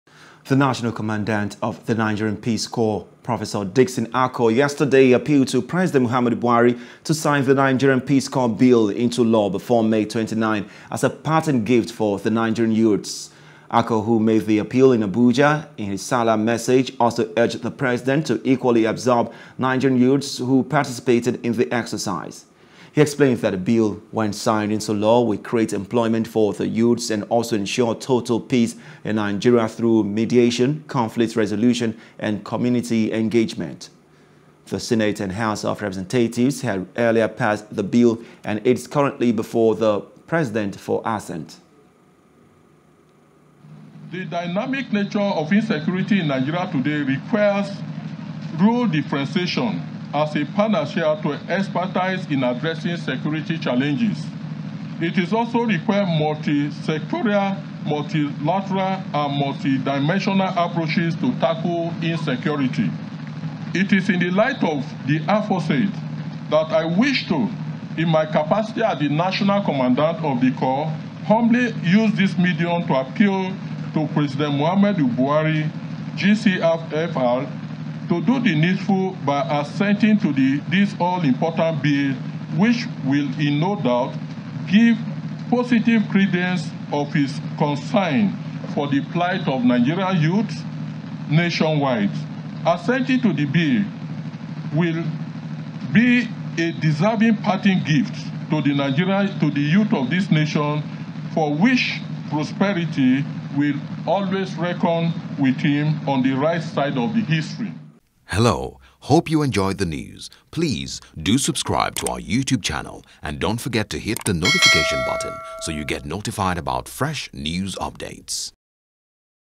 Youths: Sign Peace Corps Bill As Parting Gift , Buhari Urged
The National Commandant of the Nigerian Peace Corps, Prof. Dickson Akoh, yesterday appealed to President Muhammadu Buhari to sign the Nigerian Peace Corps Bill into law before May 29 as a parting gift for the Nigerian youths.